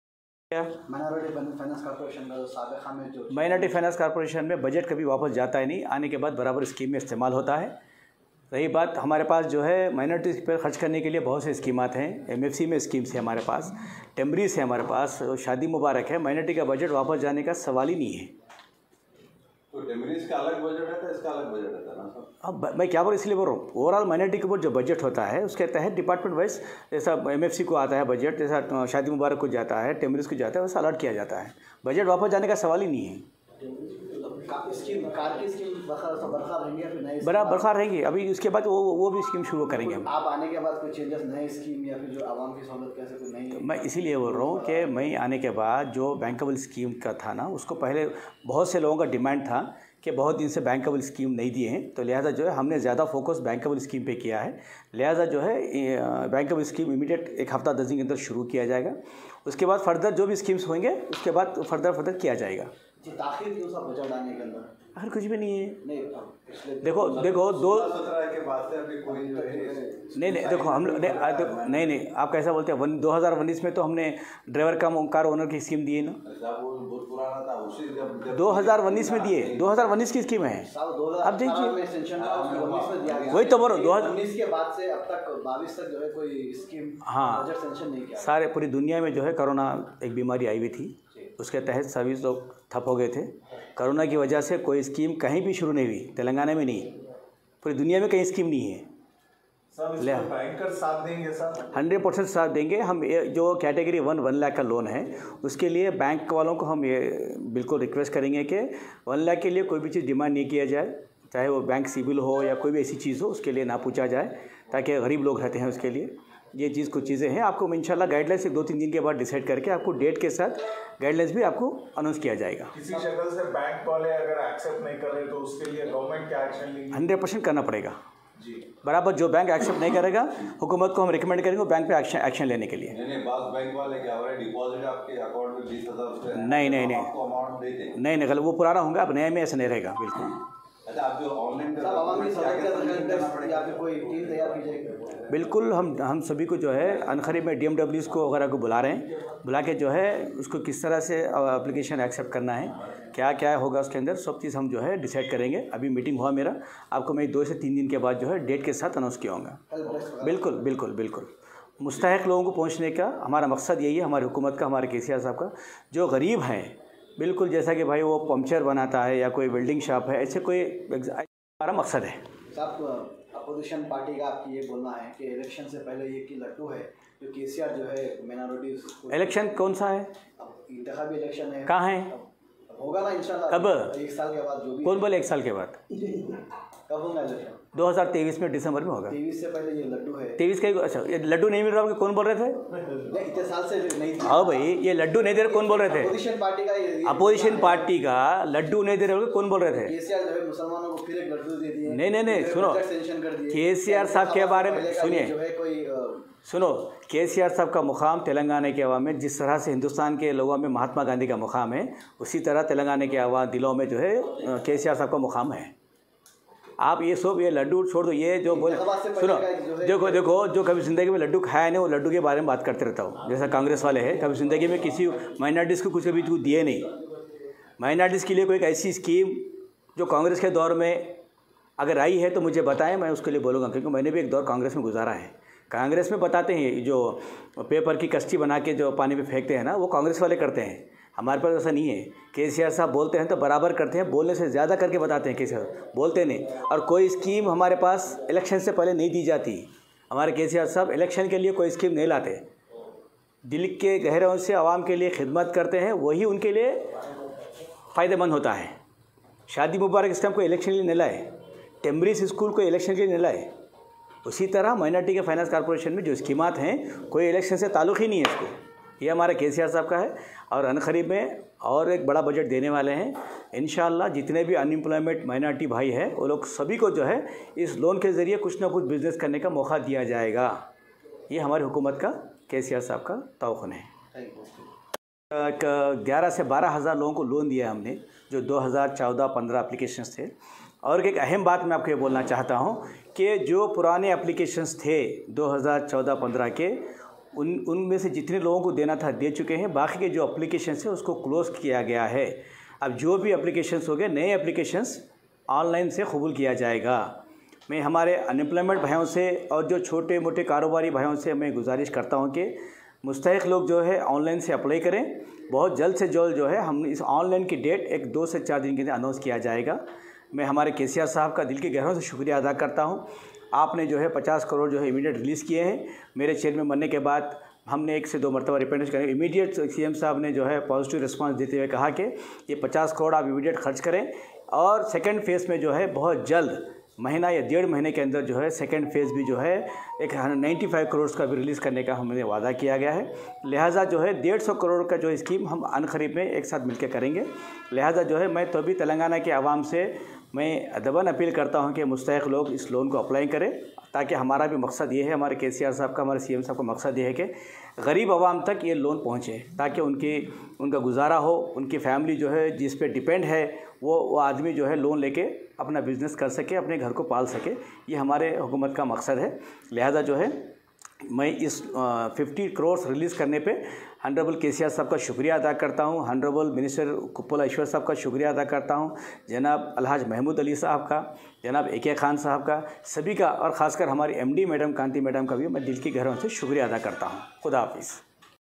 TSMFC CHAIRMAN IMTIYAZ ISHAQ TODAY ADDRESS THE MEDIA..
TSMFC Chairman Imtiyaz Ishaq today address the media, Speaking To Media he Said that Cm KCR has Announced to Restart The Loan Facilities through Bank,
 it's Have  Divided into two Categories,  one is One Lakhs, and Two Lakhs, with 80% & 70%  Budget of 150 Crs, in first Face Govt Has to Release 50 crs and In Second Phase Govt will release 100 Crs,,,,...

All the Previous Application Has been Close, has to Apply New Application through Online,...

(NEWS14..NAYYER)